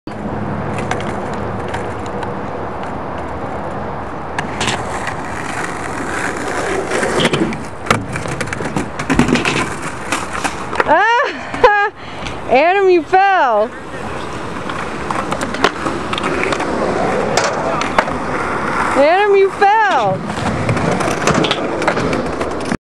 Adam's skateboard wipe out!
Adam drops in on a quarter pipe & tries to ollie on the fun box & wipes out!